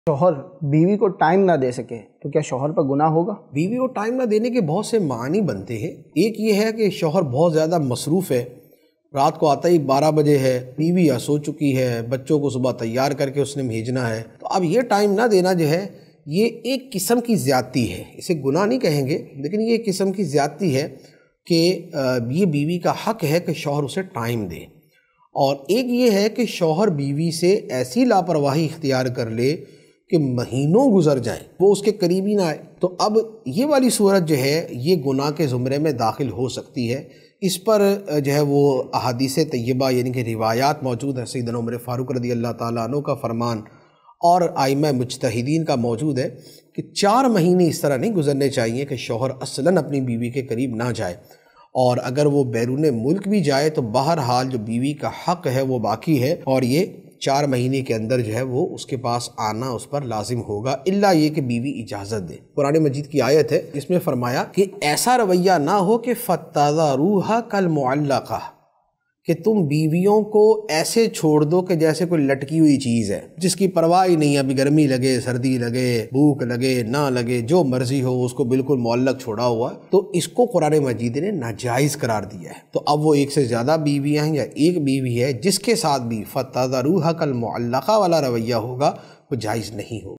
Shohar Biwi Ko Time Na De Saky to Gunahgaar Ho Ga? | Husband Wife Relationship | Mufti Qasim Attari
Topic:اگر شوہر بیوی کو ٹائم نہ دے سکے تو گنہگار ہو گا؟